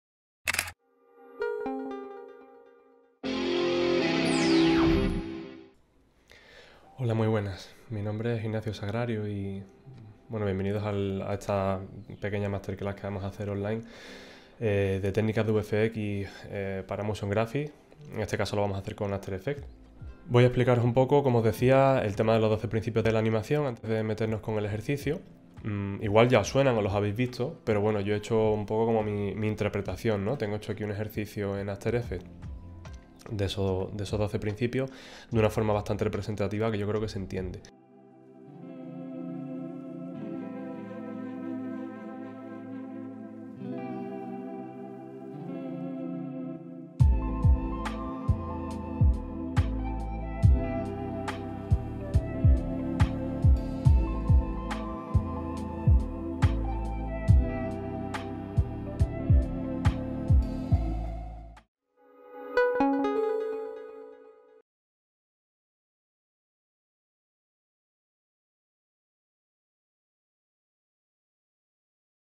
Trailer: Técnicas para la postproducción de VFX y Motion Graphics mediante After Effects
En este tutorial/workshop se explica, de manera gráfica, el funcionamiento de los doce principios de la animación (Thomas et al., 1995, Williams, 2012), y su aplicación mediante el software de motion graphics Adobe After Effects. Se explica el procedimiento de animación a partir del uso de imágenes vectoriales.

Thomas, F., Johnston, O., & Thomas, F. (1995). The illusion of life: Disney animation (p. 28). New York: Hyperion.

Williams, R. (2012). The animator's survival kit: a manual of methods, principles and formulas for classical, computer, games, stop motion and internet animators. Macmillan.

Impartido por: Ignacio Sagrario
Coordinado por: Jose Luis Rubio Tamayo
Edición y Postproducción: Jose Luis Rubio Tamayo, Ignacio Sagrario.

Sobre el Trailer:
Edición y Postproducción:
Adrián del Prado, Jose Luis Rubio Tamayo, Ignacio Sagrario.